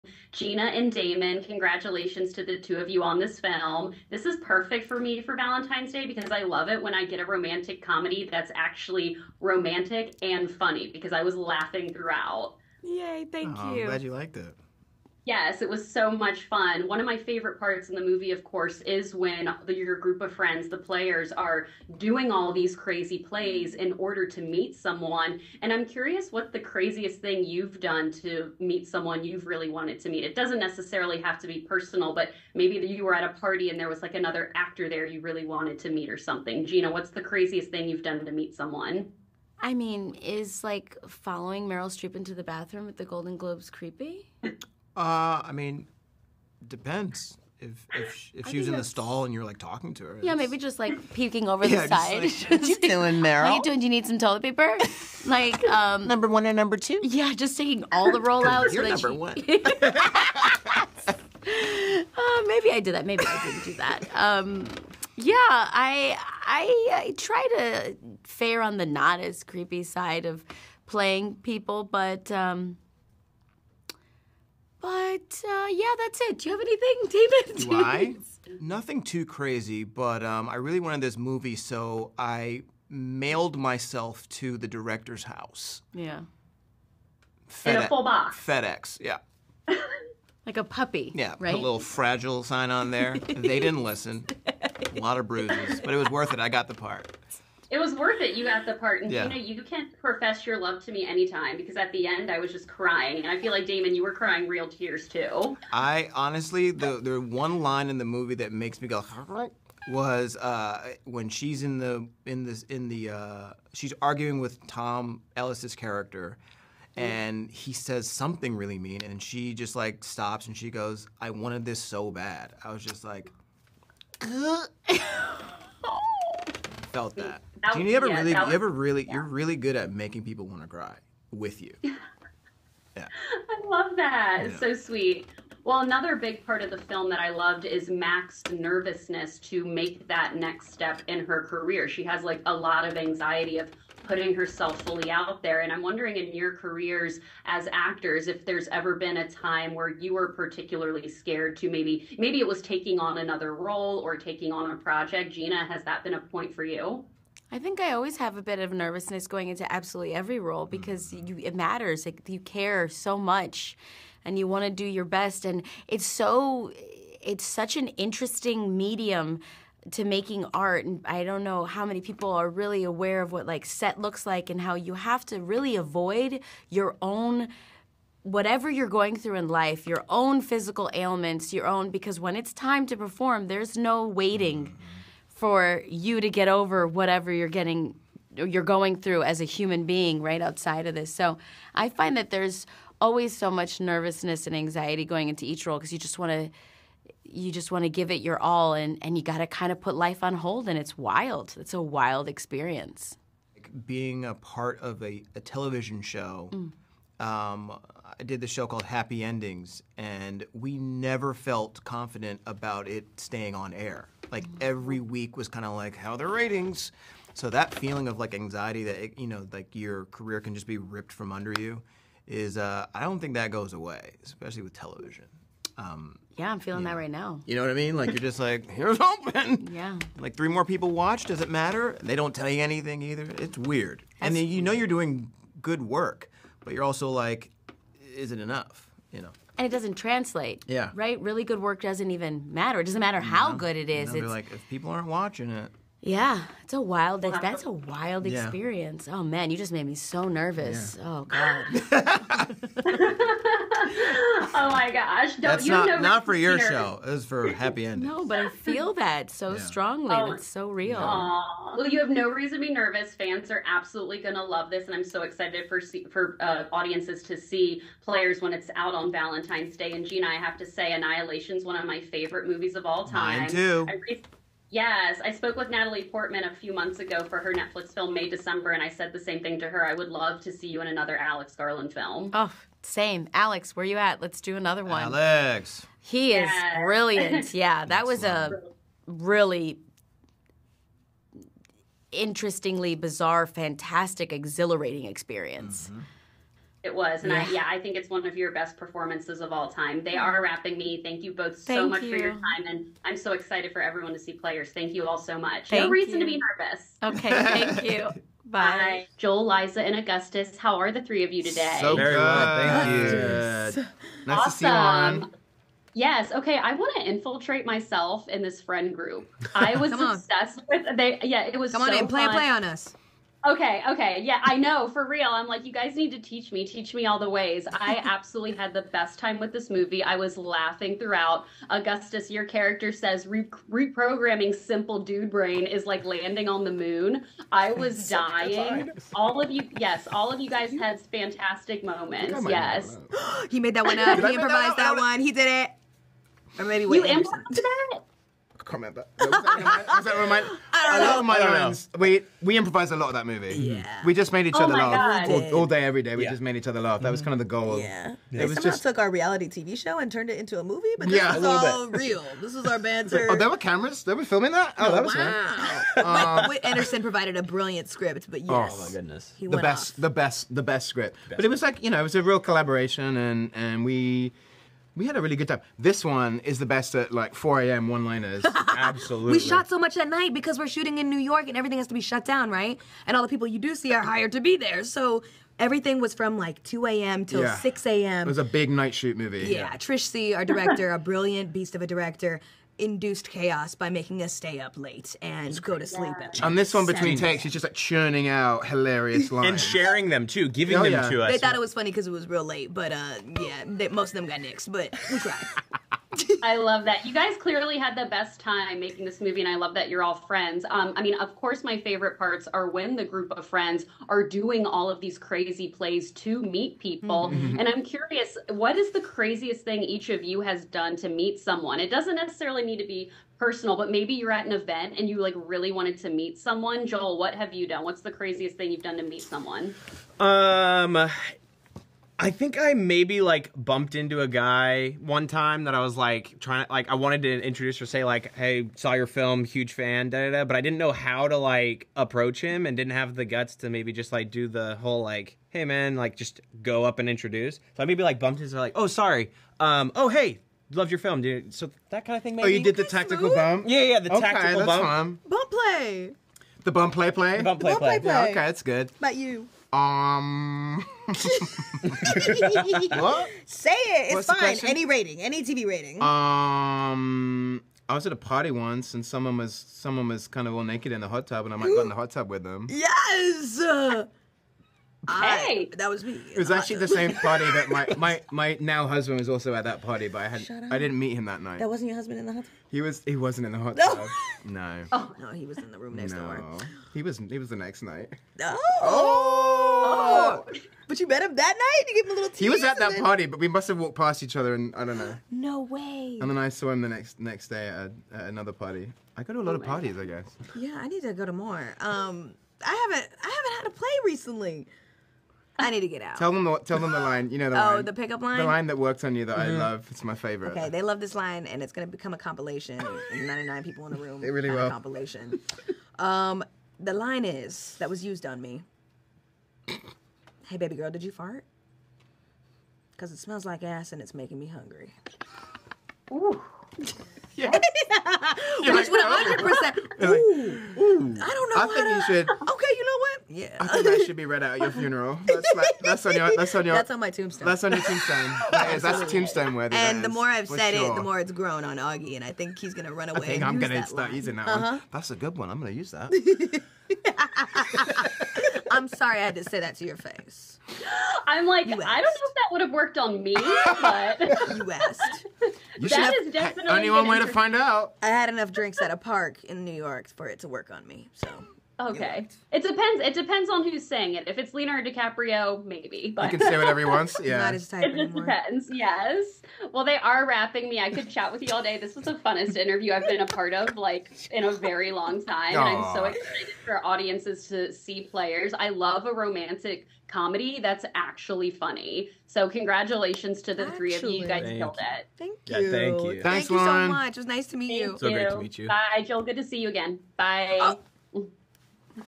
PLAYERS Cast Interview! Gina Rodriguez, Damon Wayans Jr., Liza Koshy, Joel Courtney, Augustus Prew
Lauren Veneziani (Instagram/X/TikTok: @dcfilmgirl) spoke to the cast of PLAYERS, which premieres on Netflix February 14, 2024!